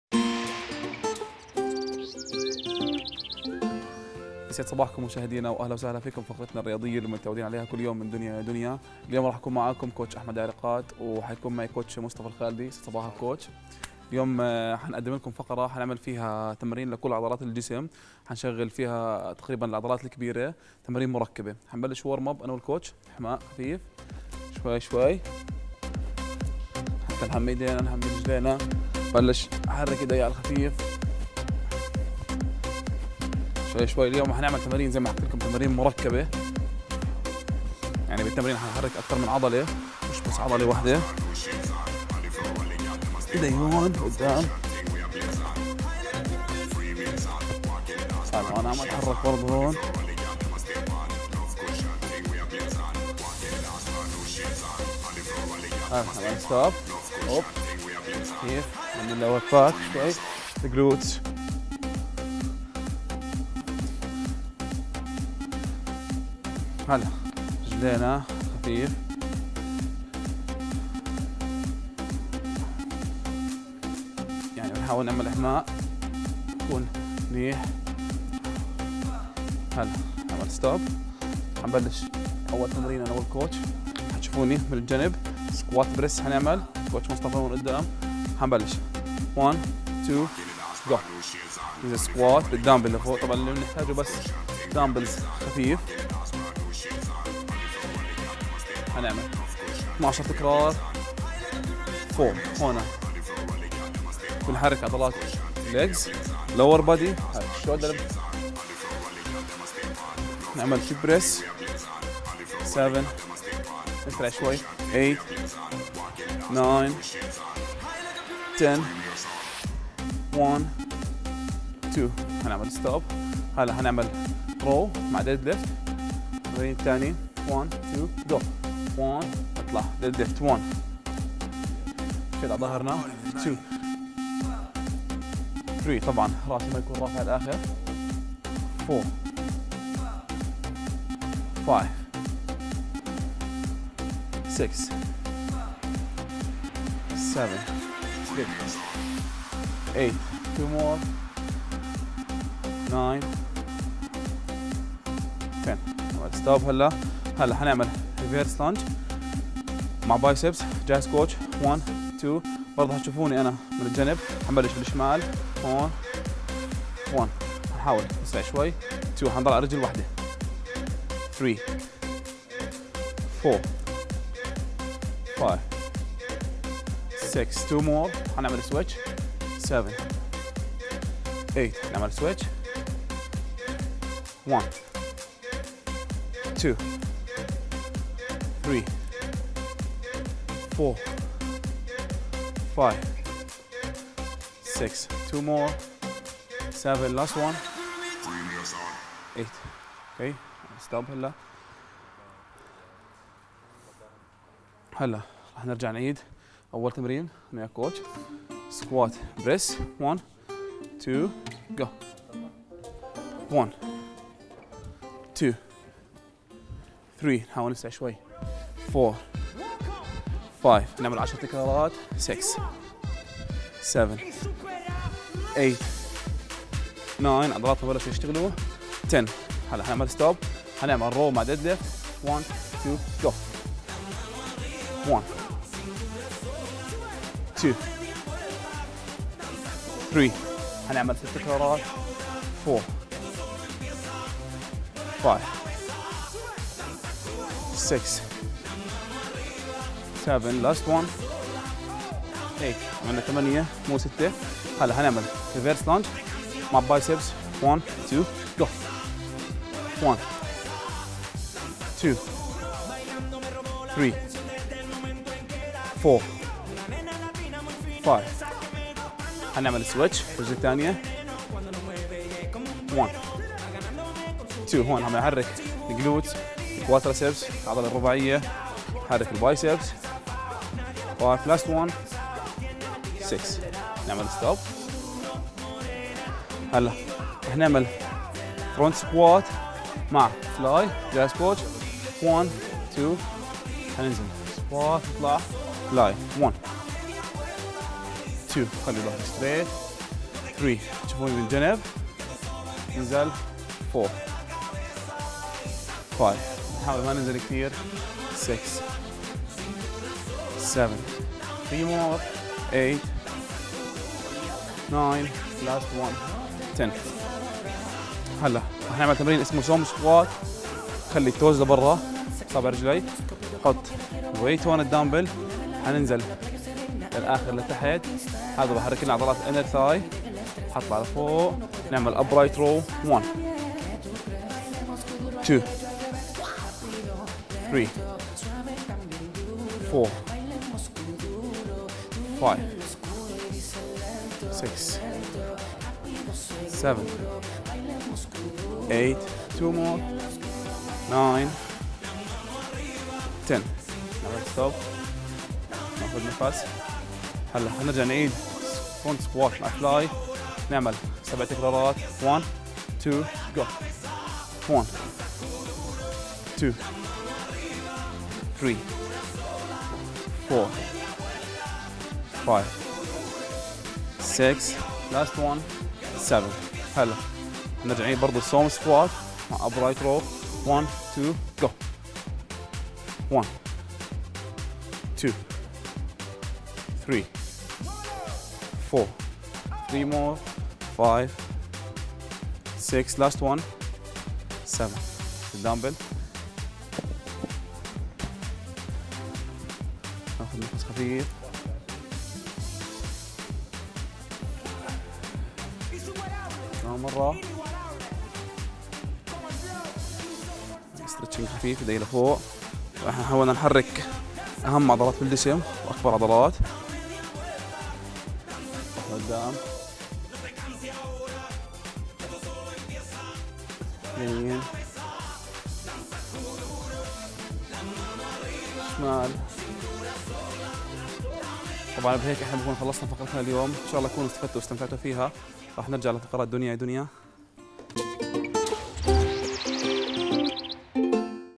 تمارين باستخدام الاوزان لتقوية العضلات - احمد عريقات - رياضة
احمد عريقات قام بعمل تمارين باستخدام الاوزان لتقوية العضلات